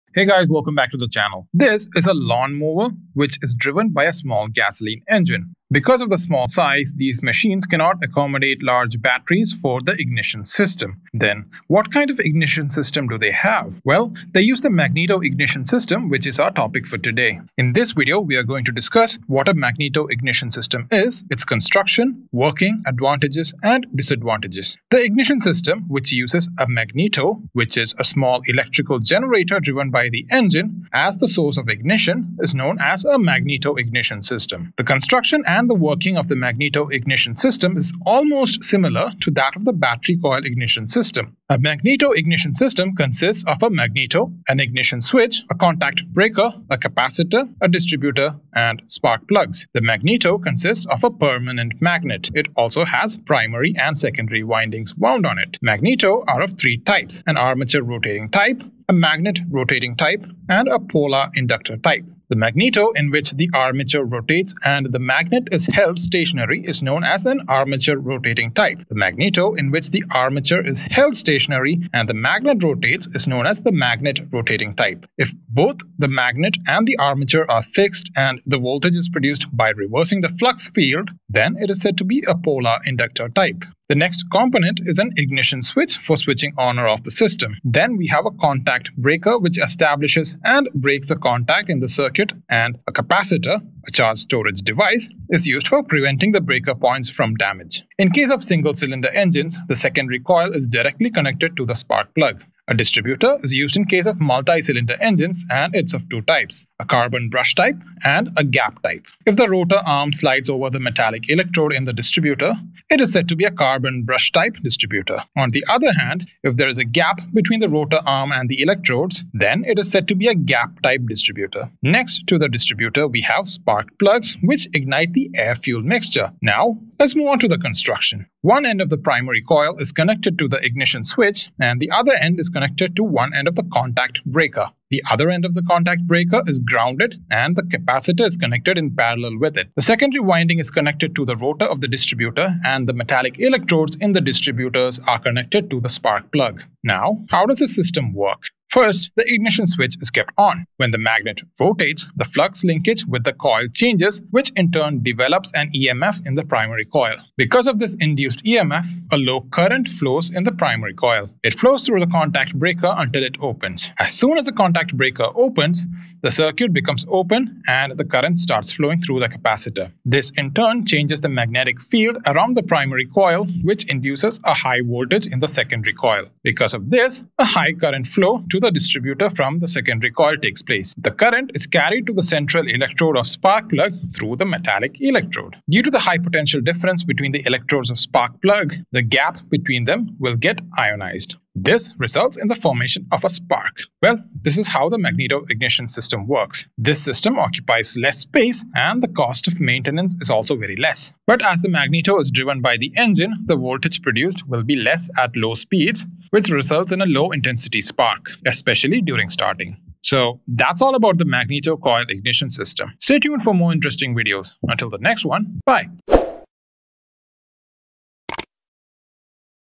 Magneto Ignition System | Skill-Lync
In one of our previous videos, we have discussed the Battery Coil Ignition System. But, some engines are too small to accommodate a battery for the ignition system. One such example is the Gasoline operated Lawnmowers.

So, do you know what kind of Ignition System they use?

They use the Magneto Coil Ignition System. Here, a magneto, an engine-driven small generator is used as an energy source of the system instead of the battery.

In this video, we have exclusively explained the construction, working, advantages and disadvantages of the Magneto Ignition System.

Magneto consists of primary and secondary windings wound over it and there is also a permanent magnet. All other components of the system and the working are similar to that of Battery Coil Ignition System.

The system has an ignition switch, a contact breaker, capacitor, distributor and the spark plugs.

When the magnet rotates, the flux linkage with the coil changes which induces an emf in the primary coil. The current due to this emf flows through the primary circuit. But, as soon as the contact breaker opens, the circuit breaks and the magnetic field around it collapses. This induces a very high voltage in the secondary coil which is used for producing the sparks.

This system requires less space and less maintenance. But, the intensity of the spark produced at low speeds will be less. This is its major disadvantage.

About Skill-Lync

Image Credits:

User Sonett72 on en.wikipedia / Copyrighted free use